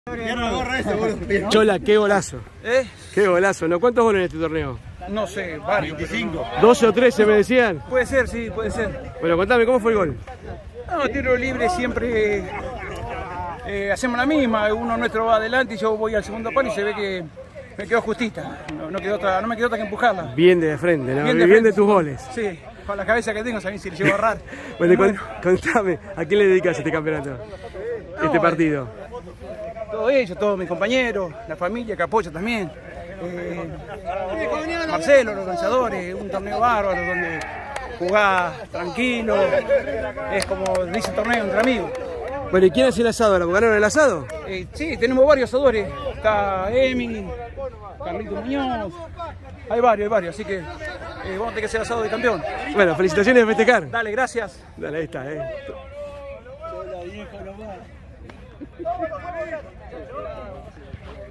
“Chola” Cisneros, goleador de la finalísima del Torneo Entre Amigos
En la tarde de este sábado se vivió una verdadera fiesta en el predio del Sindicato de Televisión donde se jugó la tercera edición del Torneo Ente Amigos en una finalísima donde no faltó nada. El “Chola” Cisneros fue uno de los goleadores del partido que puso el 1 a 0 de la victoria parcial de El Clan.

El goleador dialogo con En Boca De Todos HD y dijo que: “Fue de un tiro libre siempre hacemos la misma, yo voy al segundo palo y no me quedo otra que empujarla. Con la cabeza que tengo, mira si le voy a errar”, manifestó “Chola”. A su vez agrego que: “Le dedico esto a todos mis compañeros, a la familia que apoyan, a los organizadores. Acá jugamos tranquilos entre amigos”. En la tercera edición del torneo, Cisneros convirtió 12 goles.